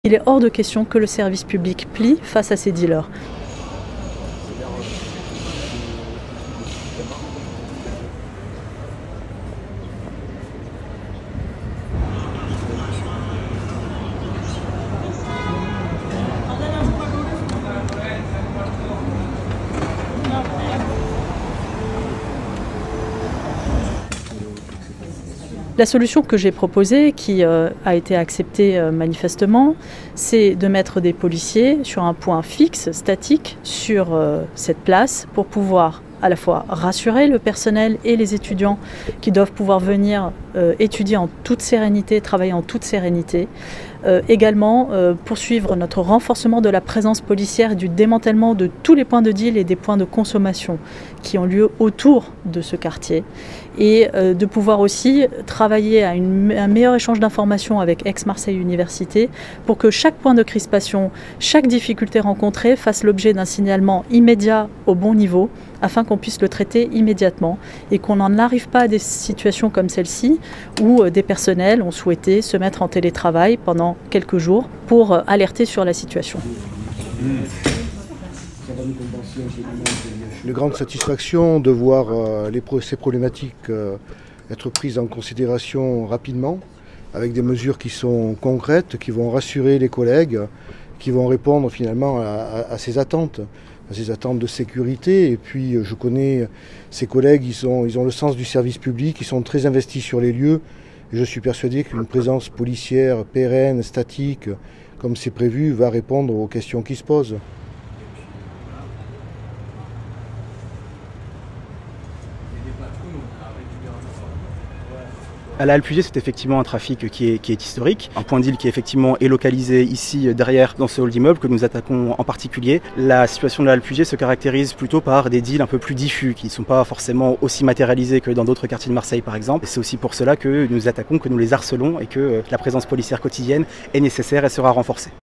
Trafic de drogue : une présence policière permanence devant la faculté de Belsunce à Marseille (1er)
Après la décision de la Faculté d’économie et de gestion mardi de fermer son site situé place de la halle Puget à Belsunce (Marseille, 1er arr.) pour une semaine dès vendredi, pour protester contre un trafic de drogue voisin, une réunion s’est tenue en Préfecture de police ce mercredi afin de trouver une solution pérenne. C’est finalement une présence policière de l’ouverture à la fermeture de la faculté qui a été approuvée par tous.

Désormais, la Préfecture de police espère que cette solution suffira à la faculté pour éviter sa fermeture.

Depuis le début du mois, dans ce secteur, 29 trafiquants de stupéfiants ont été interpelés. 84 ont été appréhendés depuis le début de l’année.